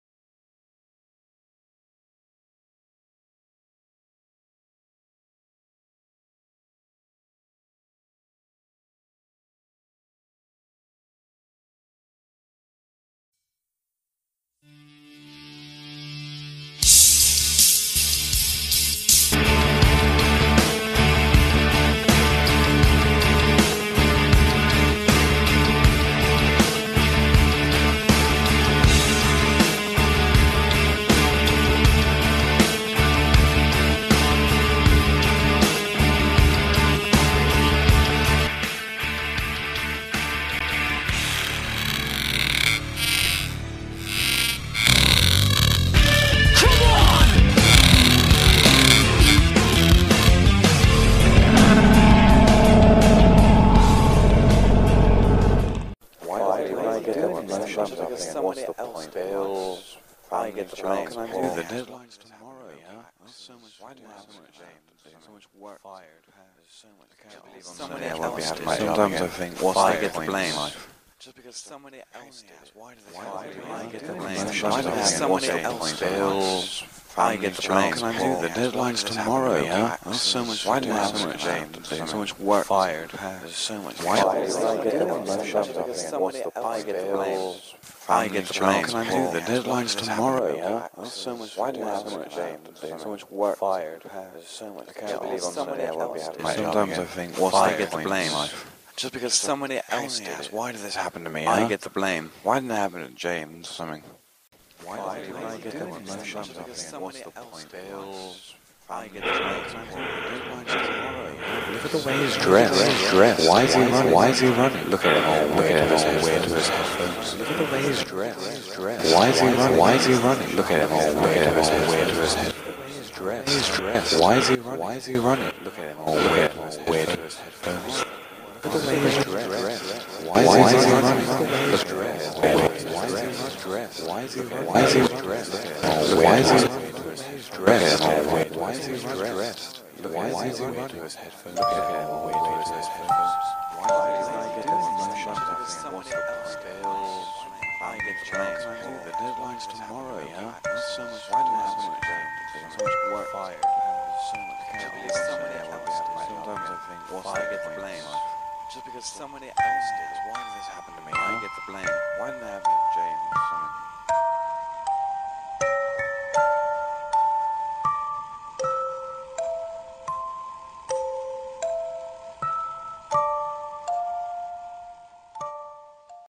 PCAD Final Major Project.
A Film Created For My Final Major Project At PCAD (Plymouth College of Art and Design) UK. This Piece Is Based On Fear And The Fear Of What Other People Think Of You.